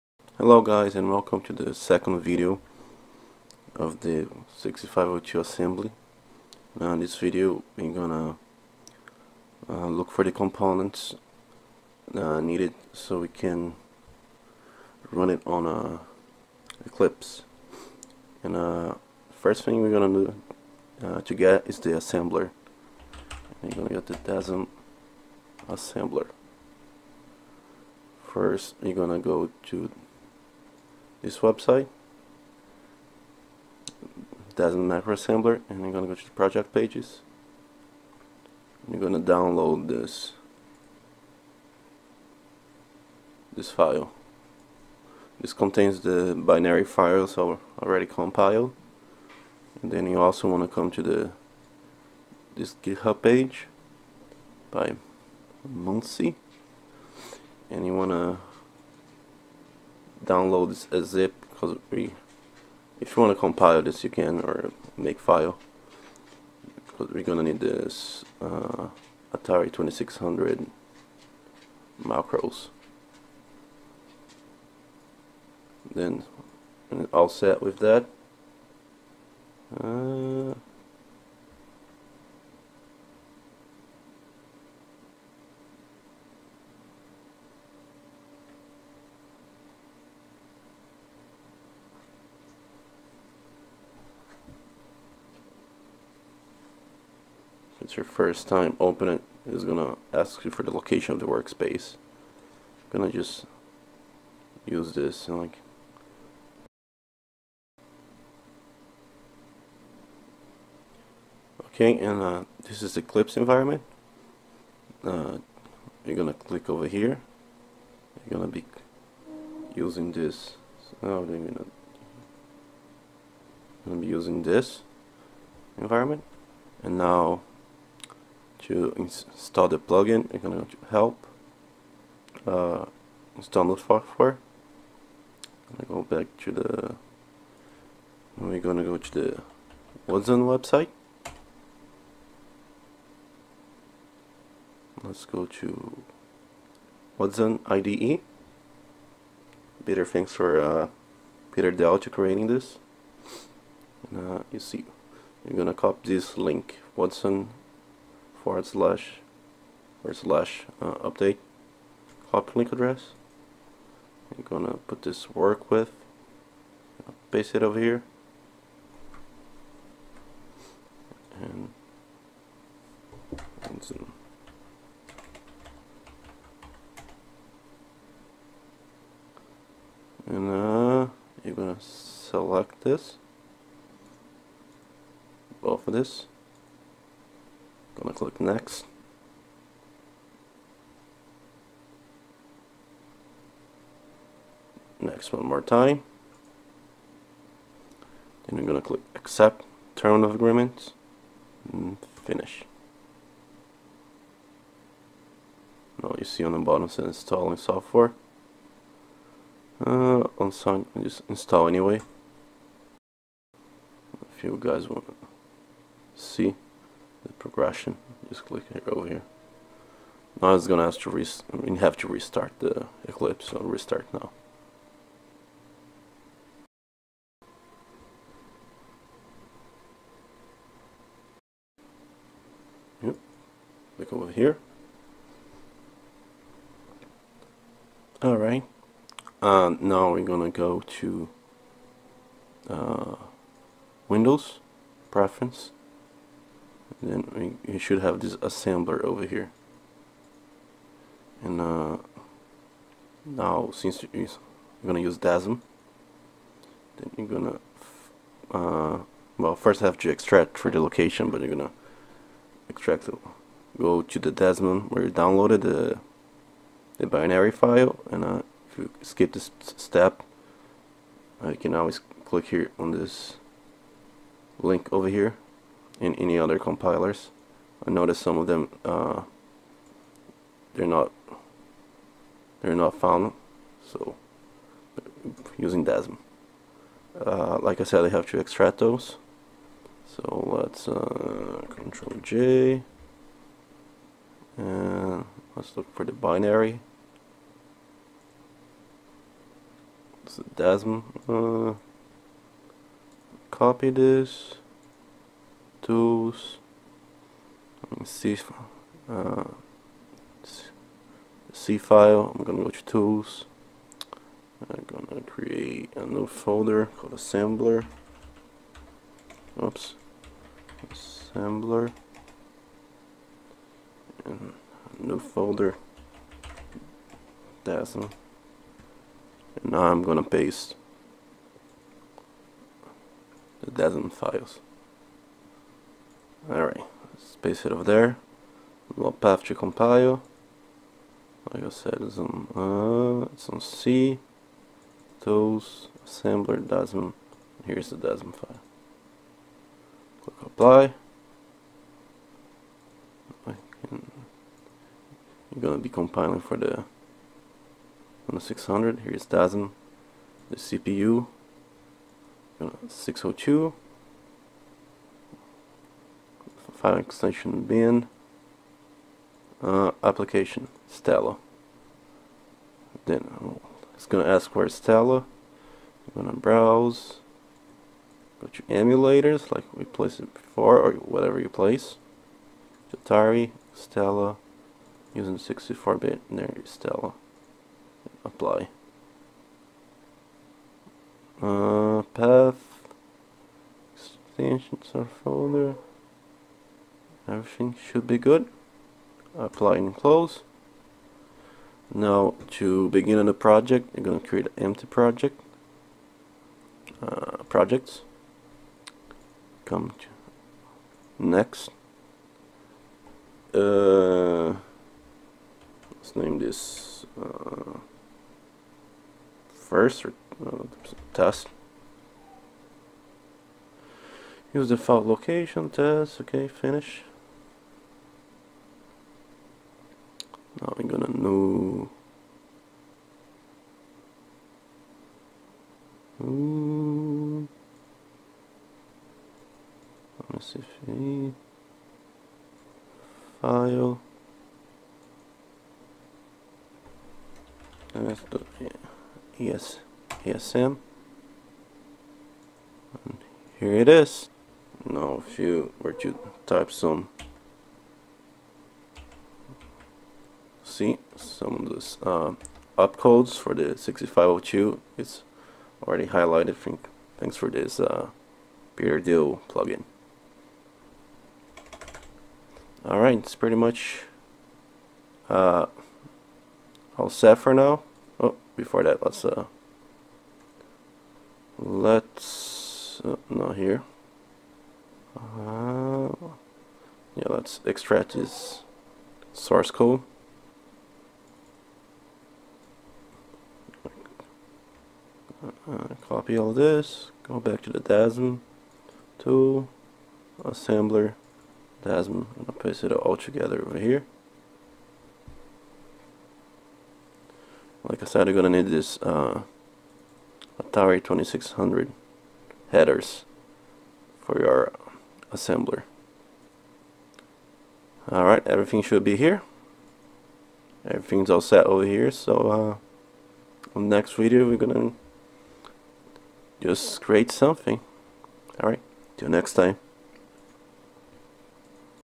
Atari 2600 Programming - Video 2: Setting Up WUDSN and DASM [Audio Improved]
In this video, I'll messly go trhough  download DASM compiler and setting up WUDSN plugin for Eclipse